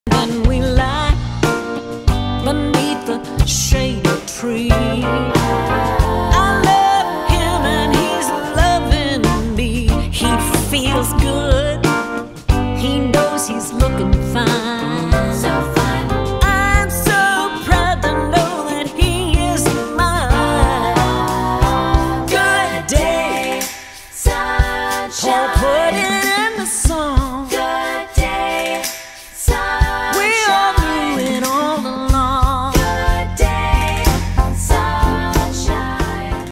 Behind the Scenes photos of recording sessions for Kate Taylor's new album!
Here’s a taste of what Kate and the band have been up to in the studio!